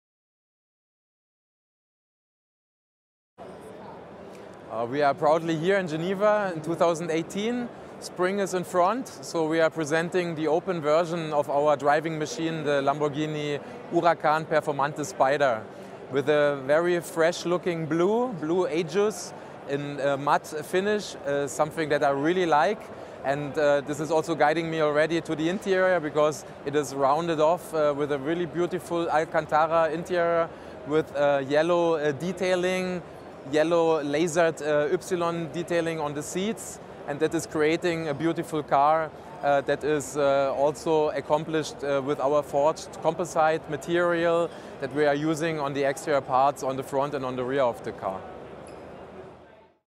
Lamborghini Huracán Performante Spyder - Interview Mitja Borkert
Automobili Lamborghini presents the Lamborghini Huracán Performante Spyder: the sublime combination of peerless technological innovation, performance and open-air driving. “The Huracán Performante Spyder takes the zenith of Huracán developments, combined with the enhanced emotion of driving a convertible,” says Stefano Domenicali, Chairman and Chief Executive Officer of Automobili Lamborghini. “The Huracán Performante already provides the most heightened feedback and emotion from road and track, and the Huracán Performante Spyder puts the driver even closer to asphalt and air, as well as the unique resonance of a naturally-aspirated Lamborghini engine.” With its inimitable 5.2 l V10 naturally-aspirated engine, the permanent four-wheel drive Performante Spyder accelerates from 0-100 km/h (0-62 mph) in 3.1 seconds, 0-200 km/h (0-124 mph) in 9.3 seconds, and with a top speed of 325 km/h matches its coupé stablemate. Braking from 100-0 km/h is mastered in 31.5 m. Like the Performante coupé, the Spyder outputs 640 hp (470 kW) at 8,000 rpm, producing 600 Nm of torque at 6,500 rpm, and with more than 70% of torque already available at 1,000 rpm. With a total dry weight of just 1,507 kg, the Spyder returns a weight-to-power ratio of 2.35 kg/hp, with weight distribution front/rear of 43/57%.